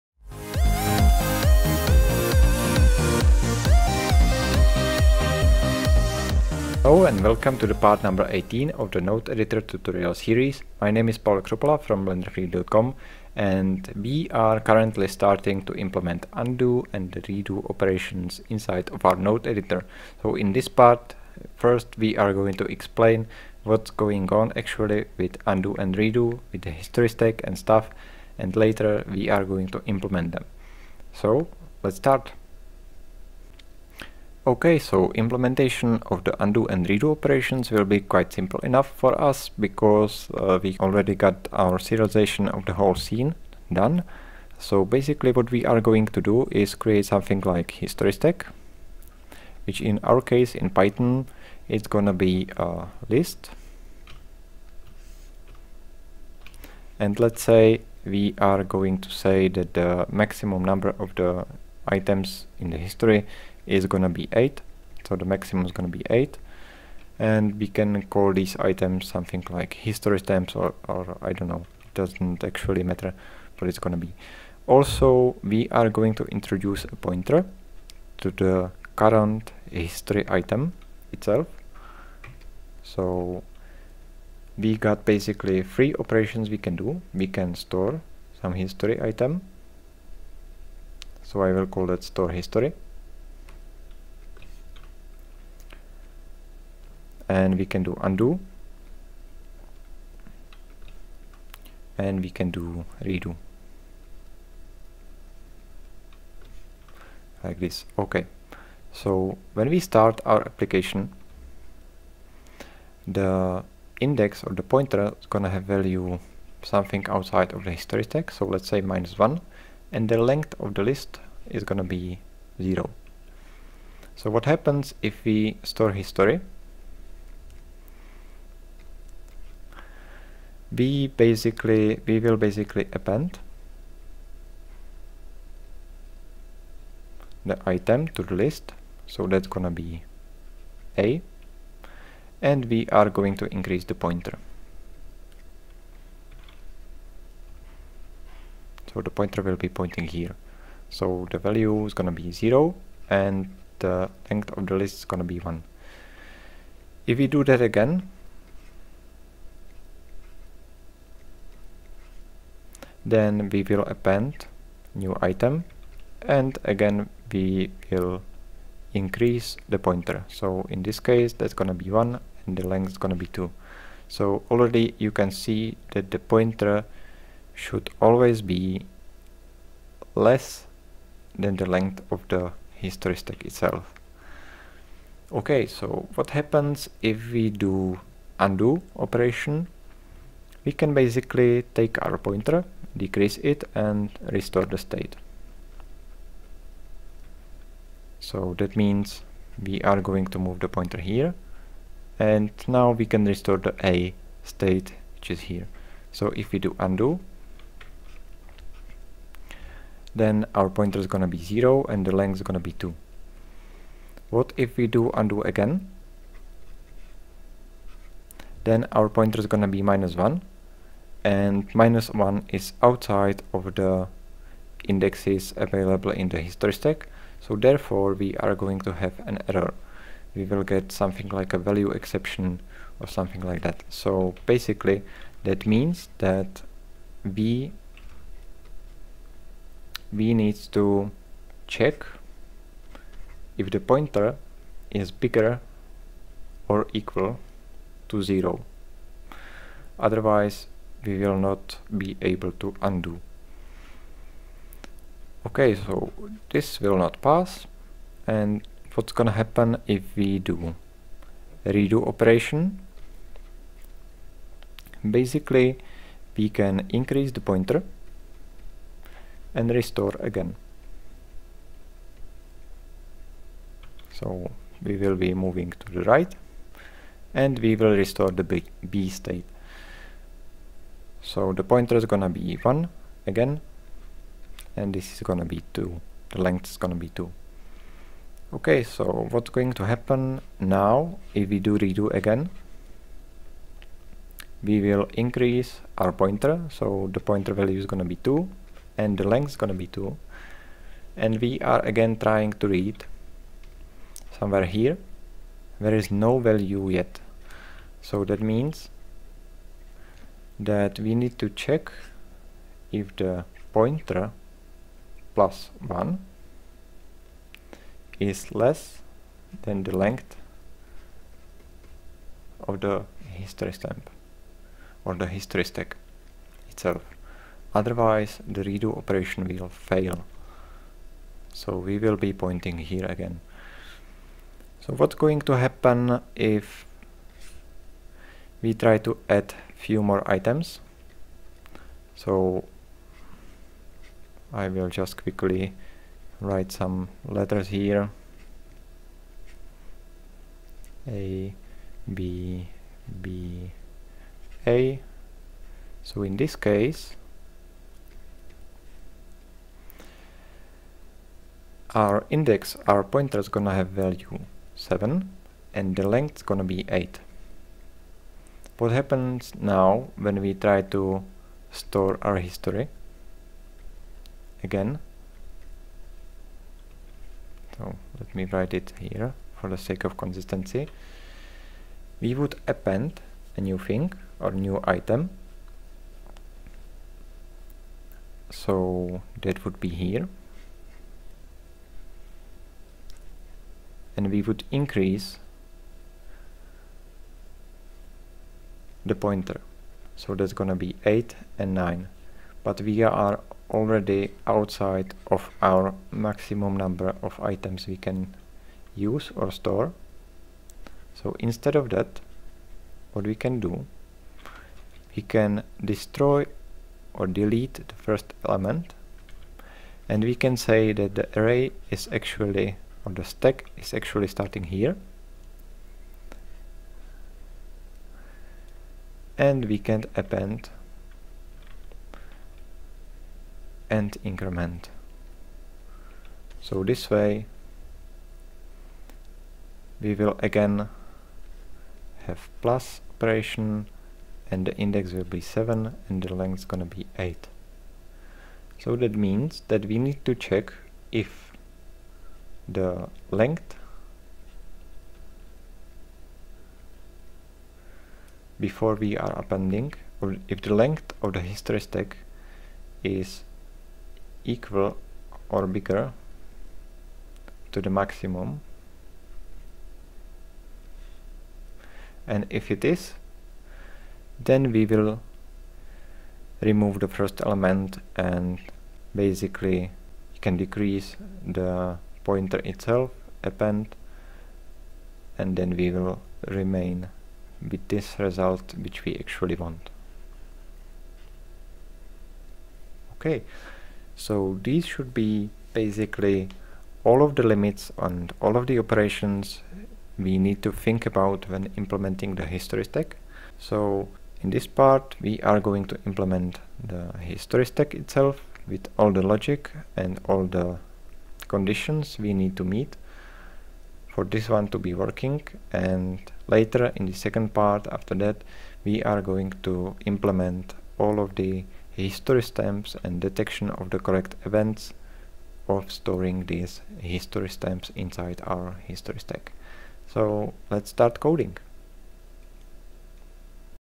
Node Editor Tutorial 18: Undo & Redo #1 - Introduction
In this Node Editor tutorial we are going to talk about the Undo & Redo functionality and how to implement it in the next part.